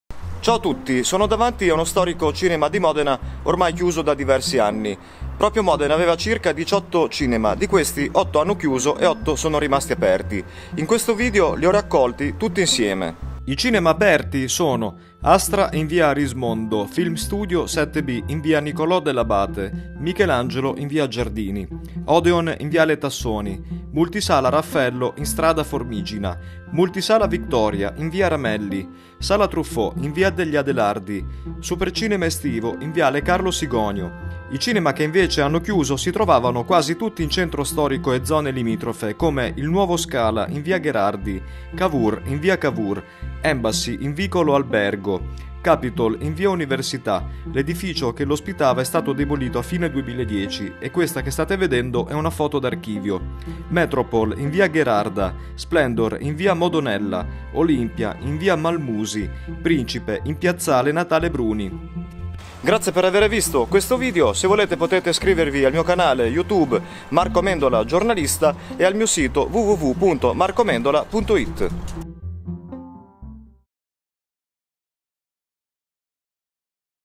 I cinema di Modena
Tutti i 16 cinema di Modena aperti e chiusi in un video. Servizio di Marco Amendola. Tutte le immagini fotografiche sono di Marco Amendola eccetto l'immagine del cinema Capitol del Fondo Gino Barbieri, Fondazione Fotografia Modena – Fondazione Modena Arti Visive.